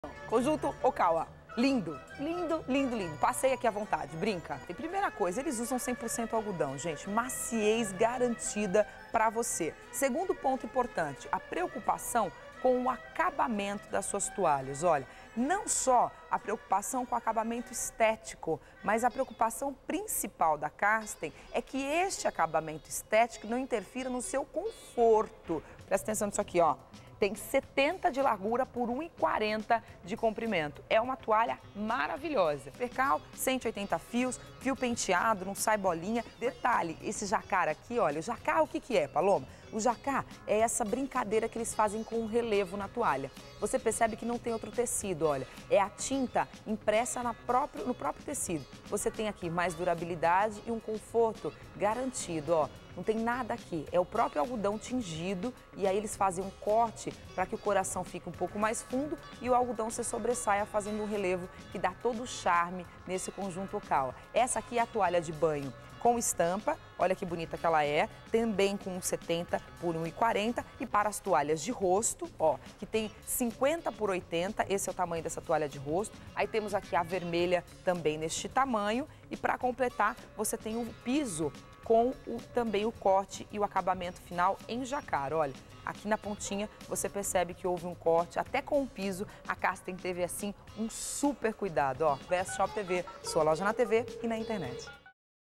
BestShopTV.com Kit de Banho Okawa 5 Peças
Pensando em você foi criado um Kit Karsten, toalhas de banho que proporcionam uma agradavél sensação através do seu toque macio, para momentos em que nada é melhor do que um banho para relaxar. BestShopTV.com